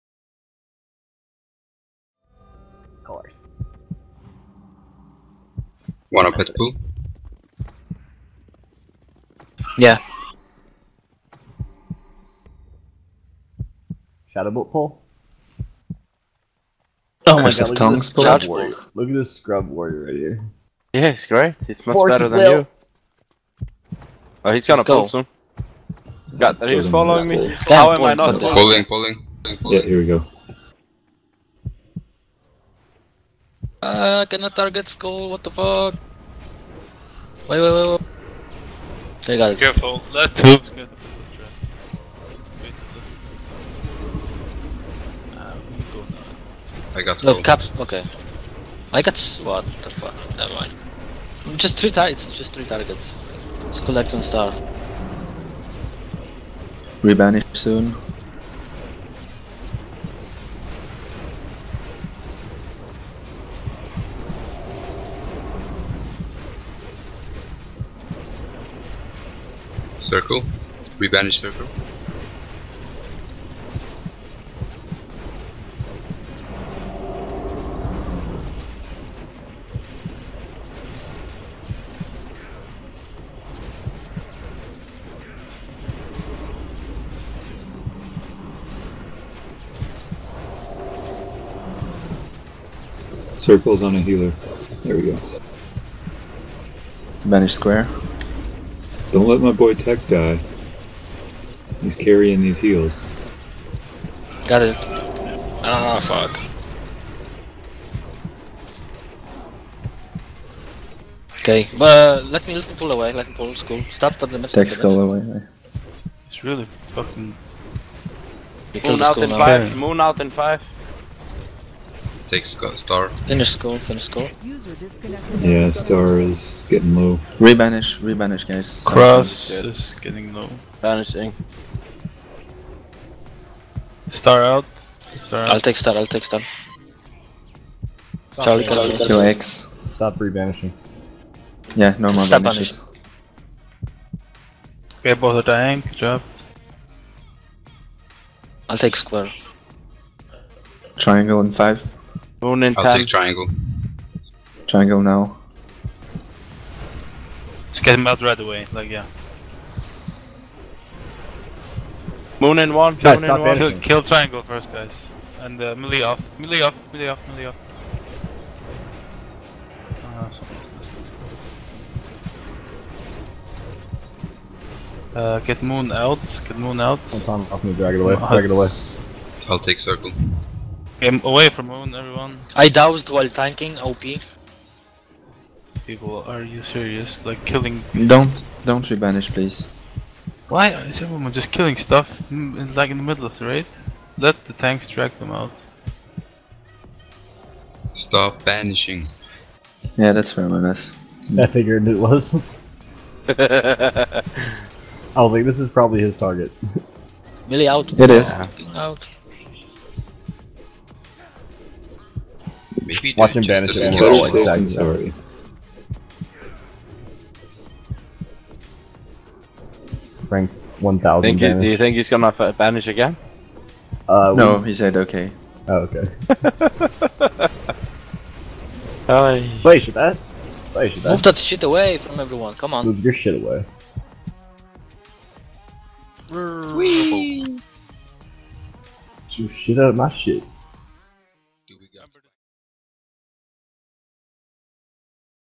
Garr - rogue PoV
Bunch of over geared mongs from Lost in Sauce killing pixels at Kronos.